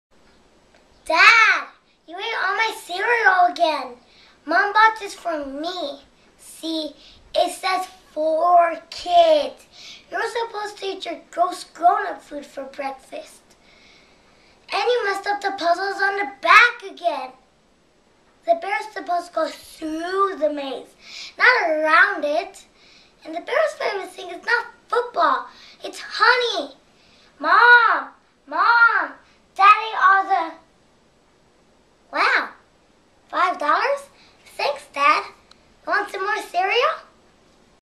Dad ate my cereal monologue
Practicing monologue for acting class.